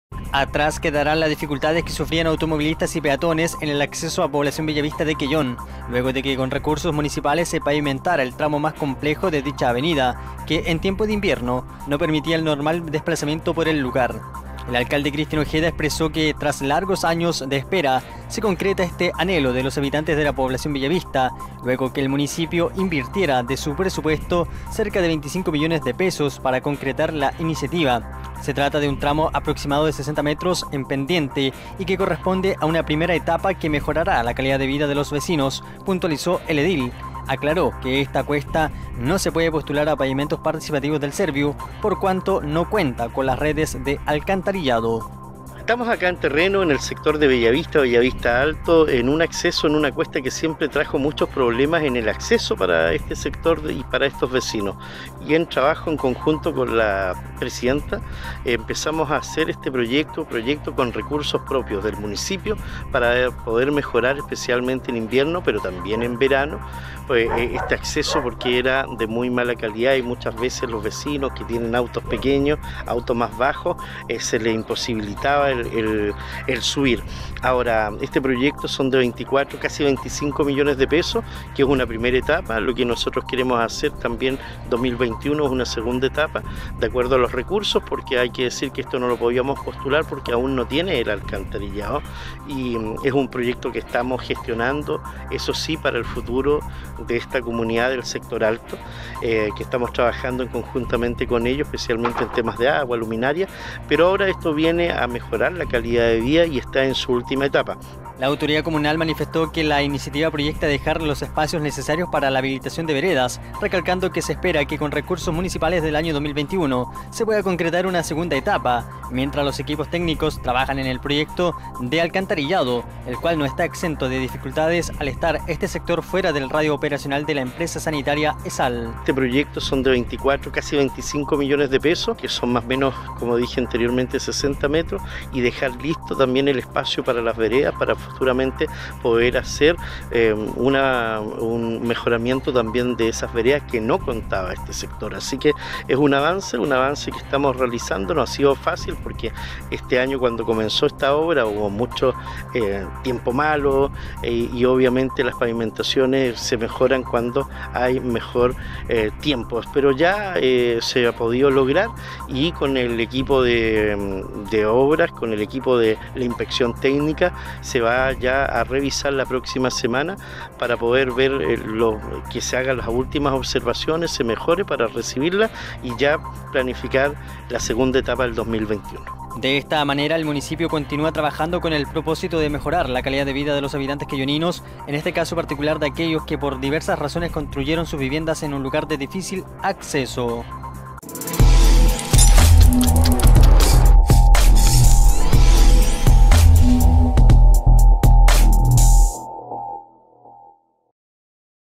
CON RECURSOS MUNICIPALES PAVIMENTAN ACCESO A POBLACIÓN BELLAVISTA EN QUELLÓN
Bellavista_Alto | Atrás quedarán las dificultades que sufrían automovilistas y peatones en el acceso a Población Bellavista de Quellón, luego que con recursos municipales se pavimentara el tramo más complejo de dicha, que, en tiempo de invierno, no permitía el normal desplazamiento por el lugar.

Vea el informe completo de nuestro departamento de prensa